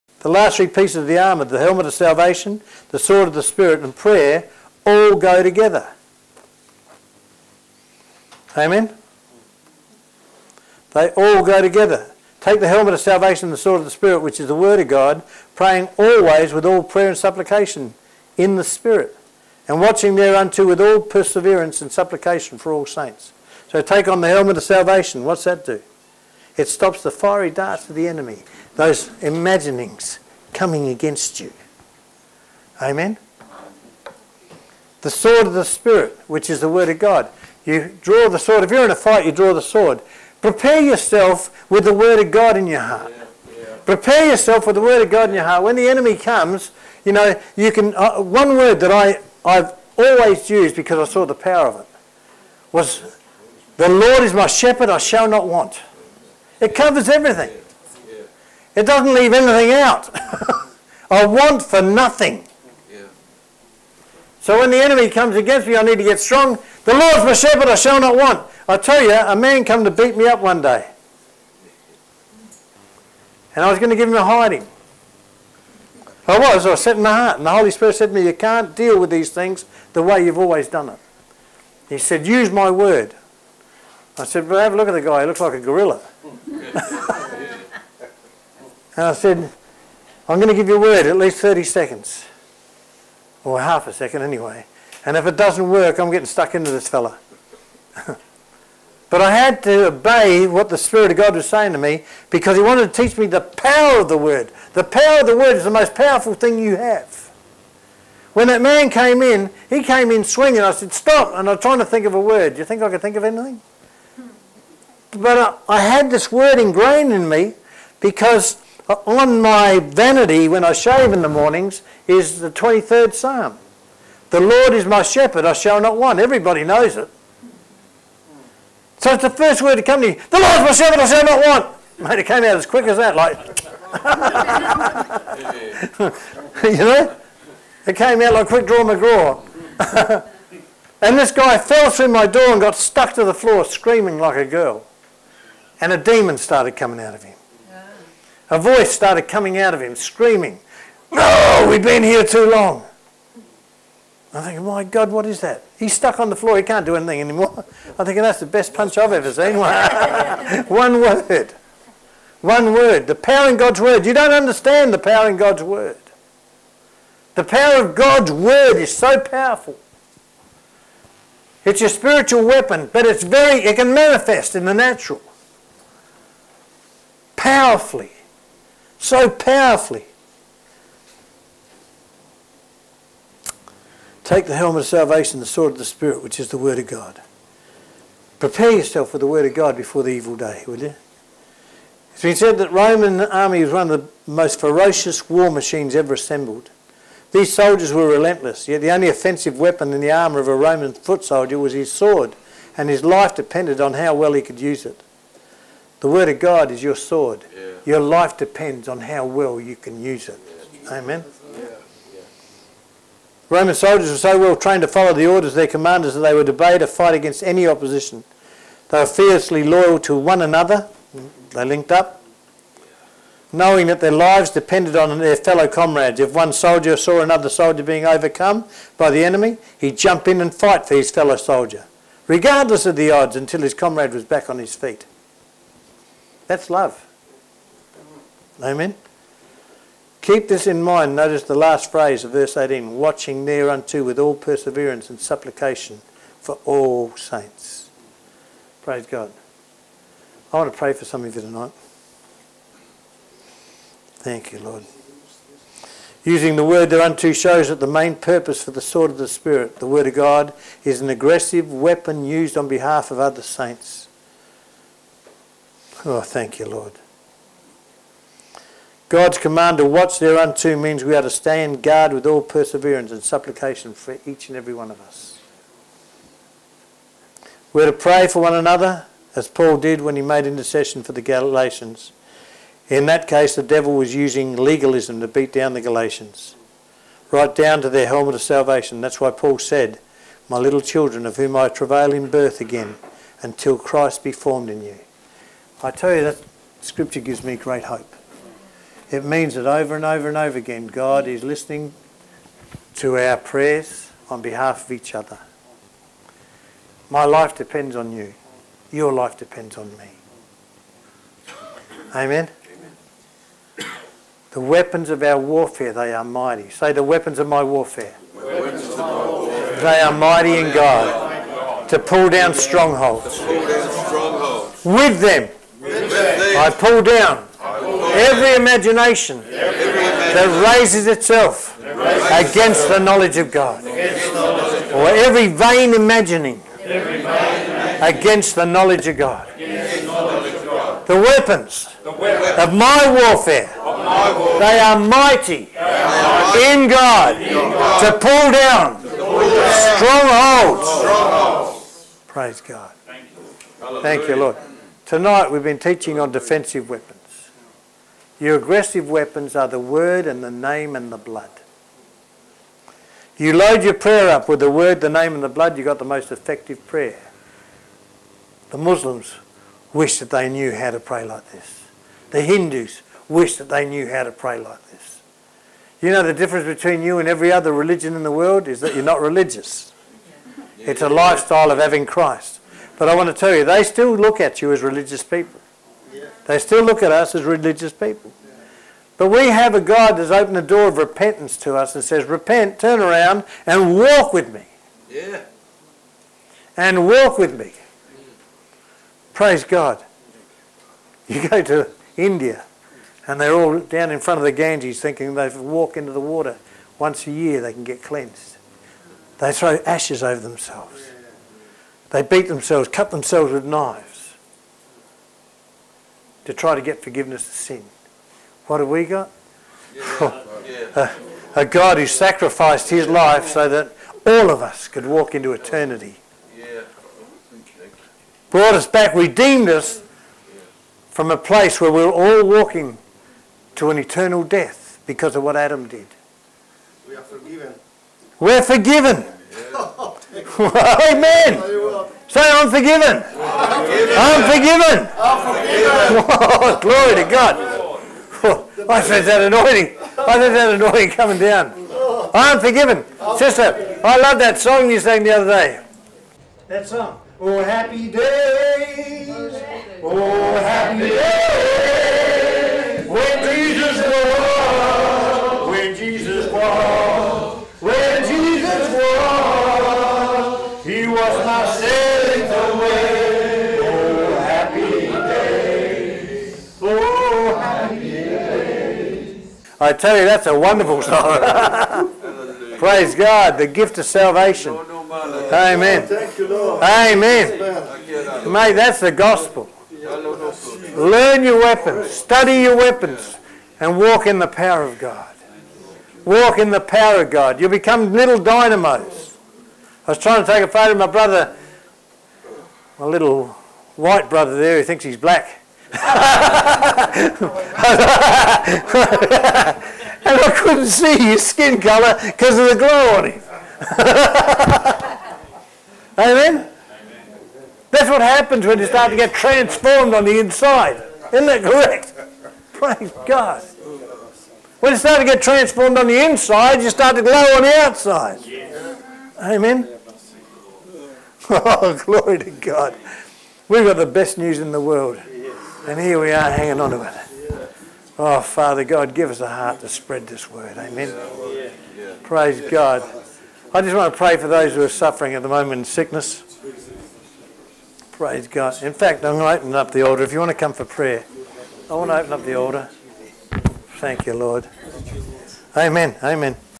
Weapons of Spiritual Warfare
Raf Shaw shares on the weapons of warfare and the power of God's word.Ephesians 6: 10-18